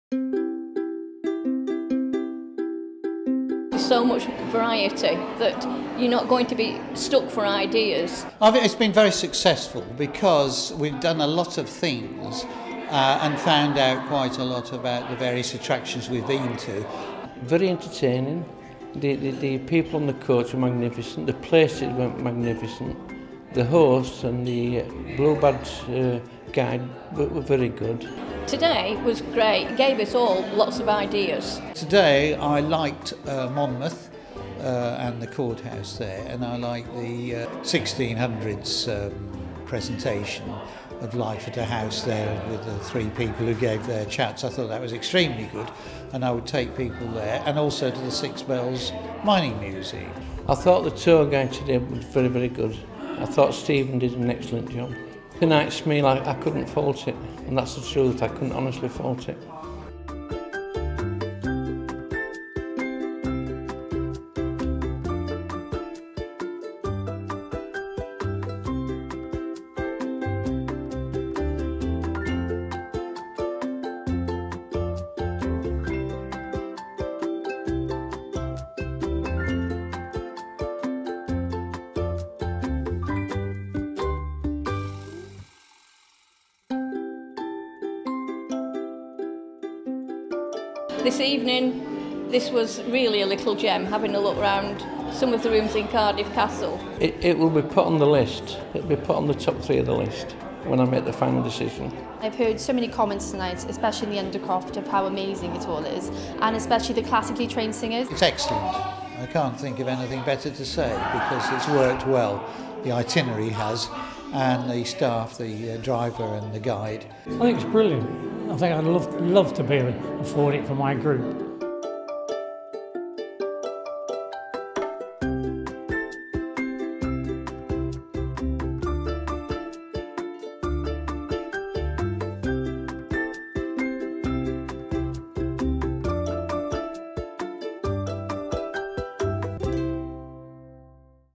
Southern Wales Familiarisation Trip Showcase | Great for Groups Magazine
In October 2016, Great for Groups Magazine (formerly The Pass) and Southern Wales Tourism hosted 25 group travel organisers on a three day familiarisation trip taking in the best attractions for groups across South Wales.

Camera and production: Chris Dicks
Copyright: Metropolis Multimedia
unauthorised usage not allowed.